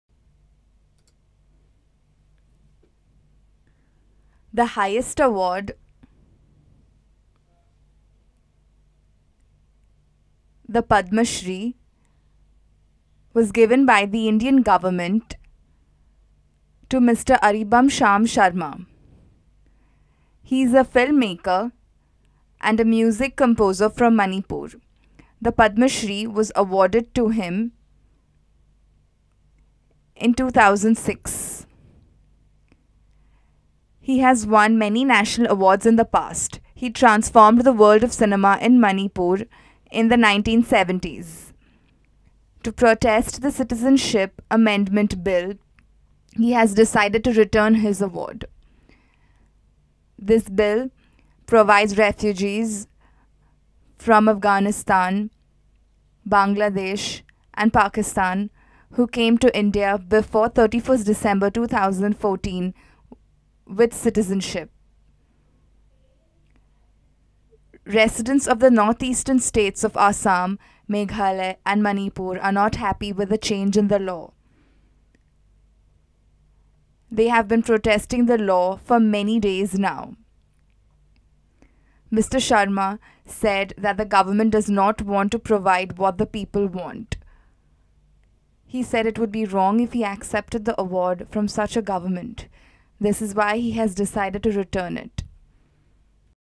Manipur Film-Maker Returns His Padma Shri Award
Film-Maker Aribam Syam Sharma returns his Padma Shri to protest the Citizenship [Amendment] Bill, 2016.